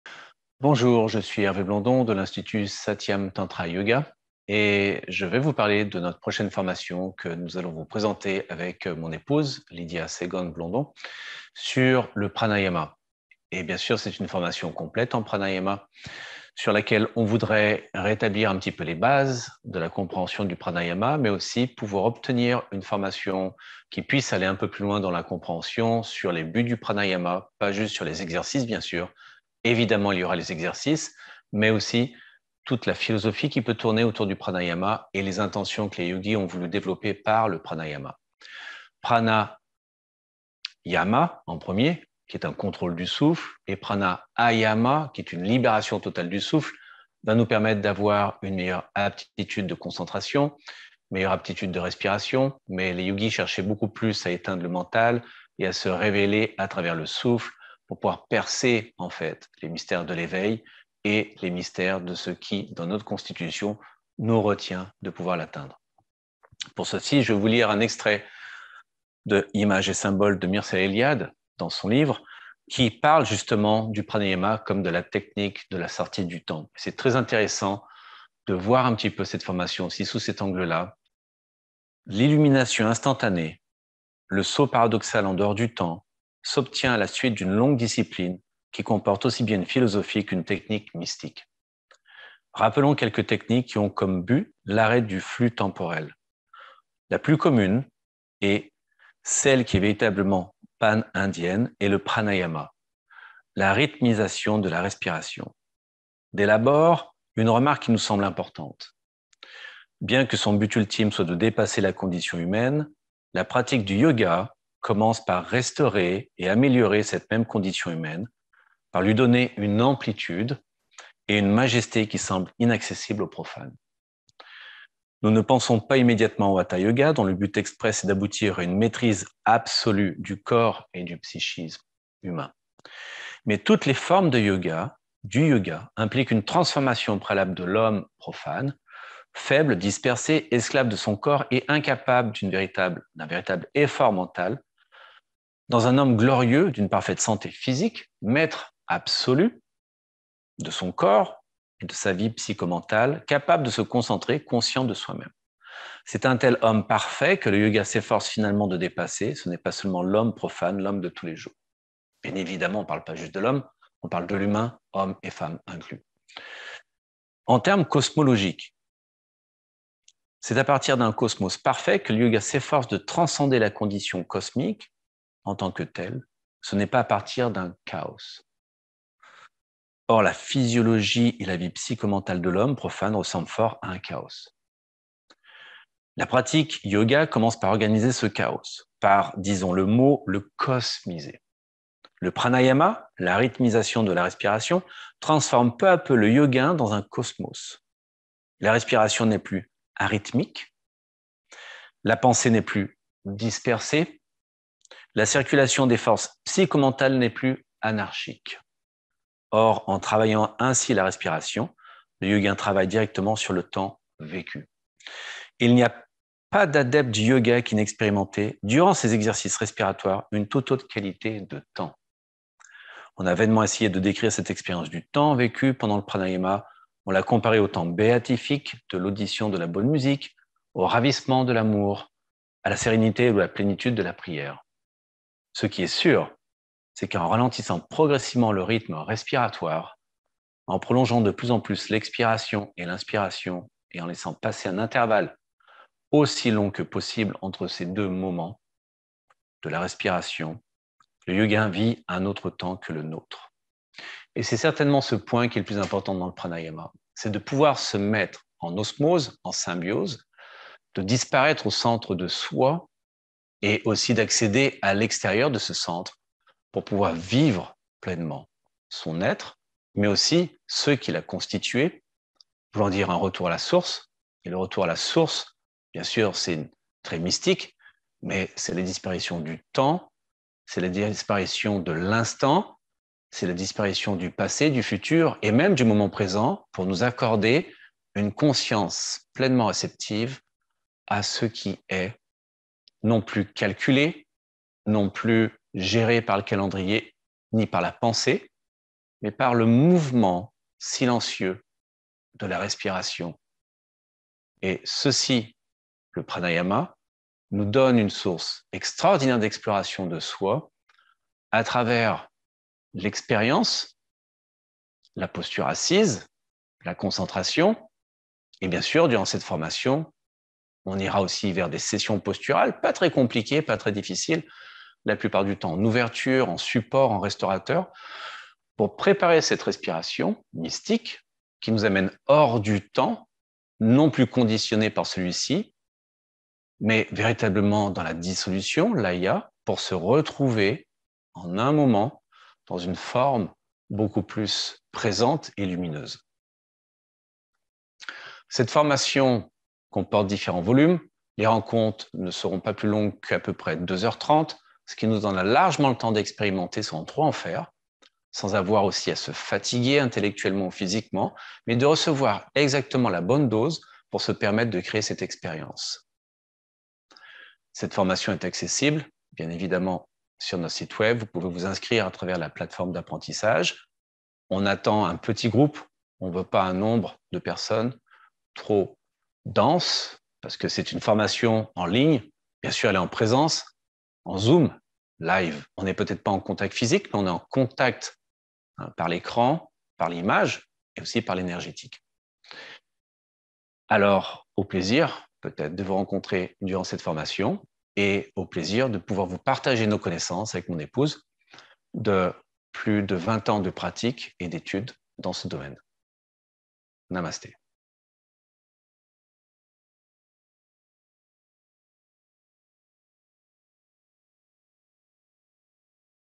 Présentation formation en pranayama
Notre formation complète et uniquement en pranayama débute ce mercredi 25 mai.